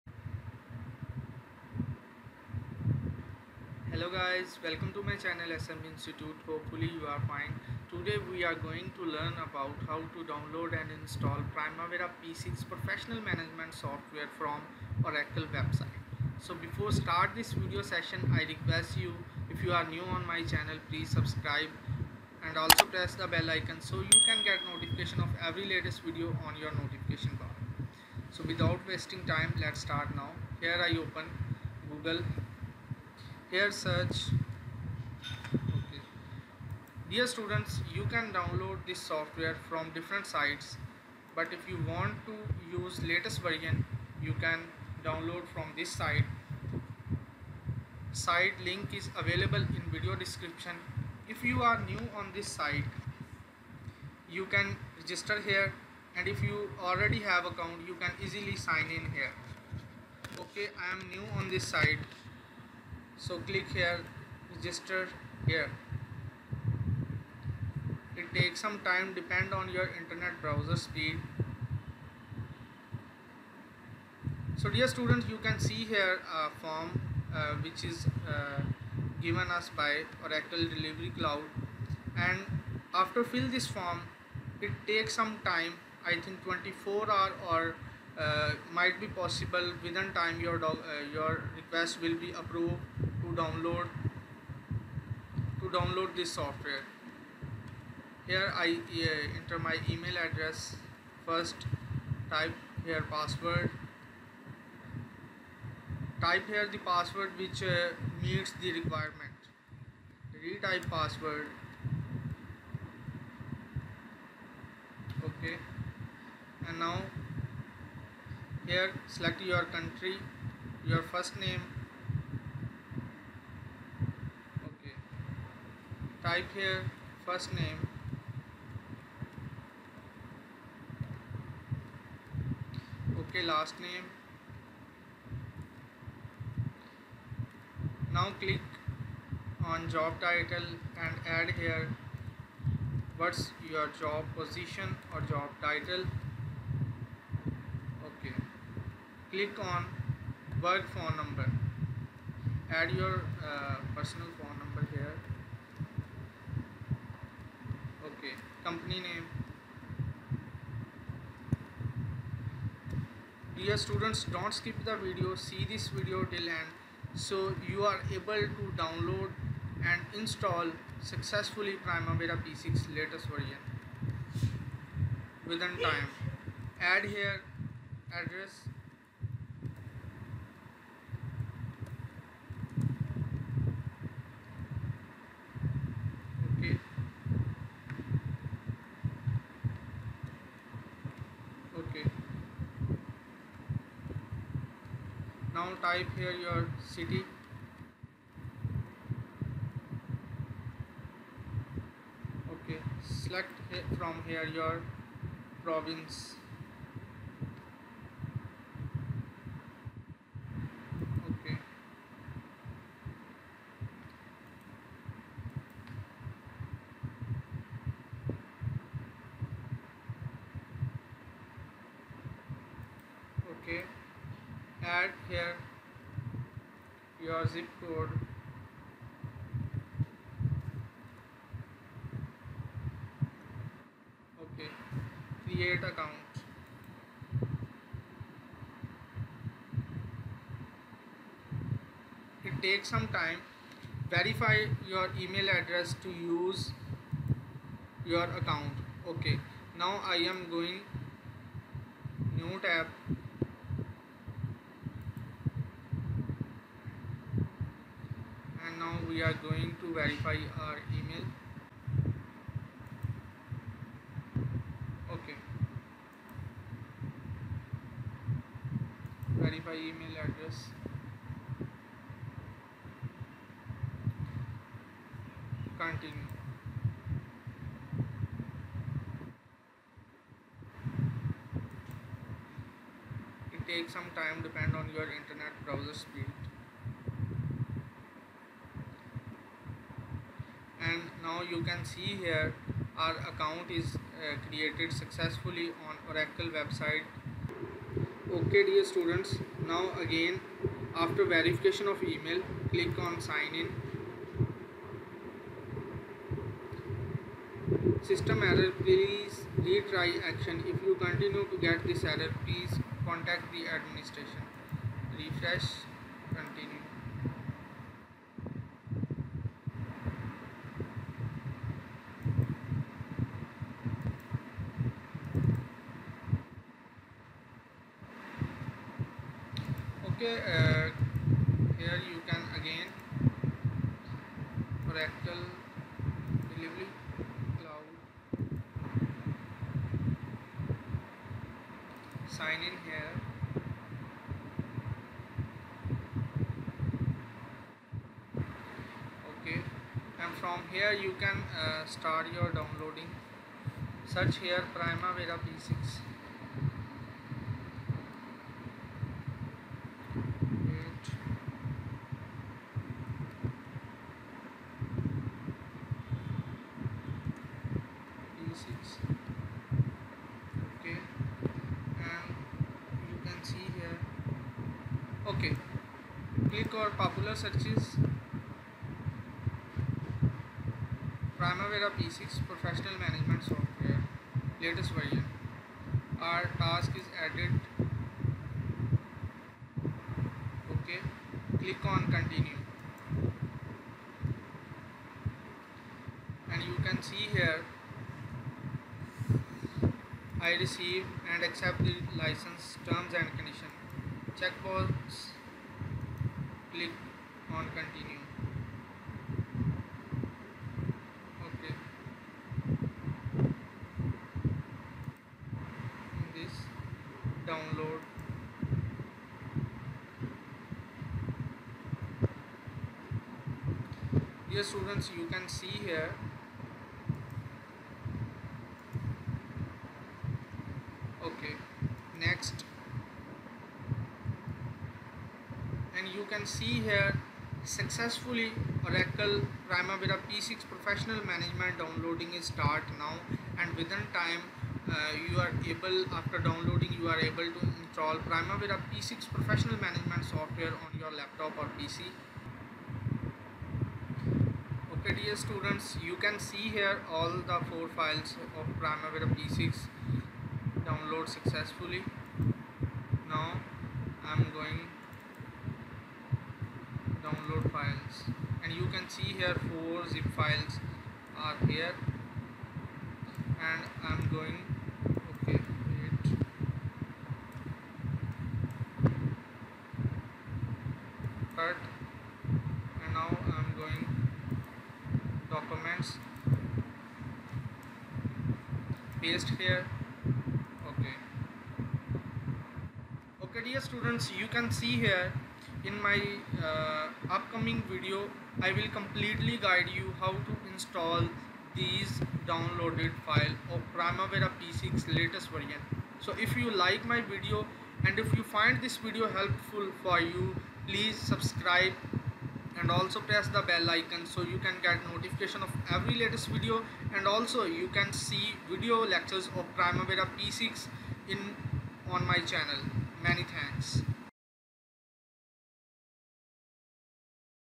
How to Downlaod Primavera P6 Free||Primavera P6 Download Free
Primavera P6 Download Free/How To Download Primavera P6 Free.

Dear Students,In this video I will completely guide you how download Primavera P6 from Oracle deliver cloud.I will guide you step by step.If you face any problem don't forget to comment.I am on the other side to answer your Questions?

Many Thanks!